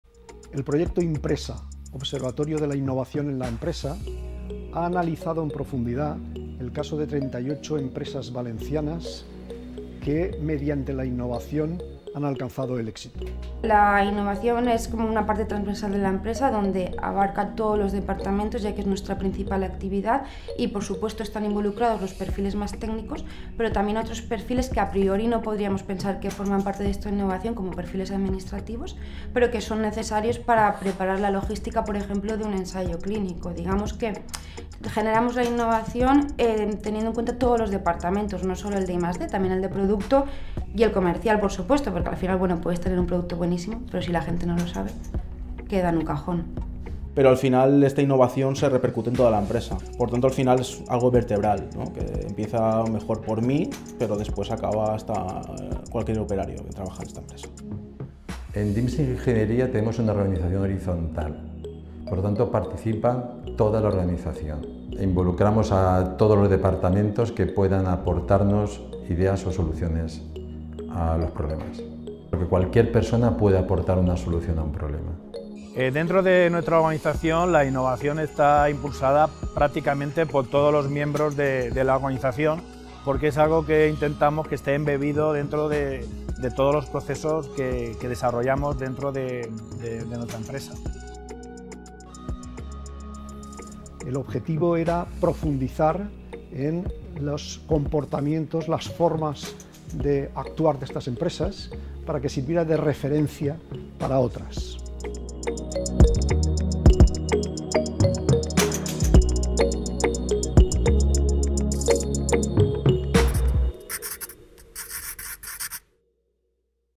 INNpresa, Observatorio de Innovación en la Empresa (Promo 2)
Para impulsar la cultura de la innovación, la Fundació Parc Científic Universitat de València (FPCUV) ha desarrollado, con el apoyo de la Agència Valenciana de la Innovación (AVI), el proyecto INNpresa; Observatorio de Innovación en la Empresa, para identificar un grupo de empresas del ámbito de la Comunitat Valenciana que comparten su alto nivel de innovación y su elevada competitividad derivada de la innovación, para poder explicitar las buenas prácticas en las que se basan, así como para elaborar una hoja de ruta que permita activar un cambio cualitativo y cuantitativo en otras empresas y organizaciones de la economía valenciana.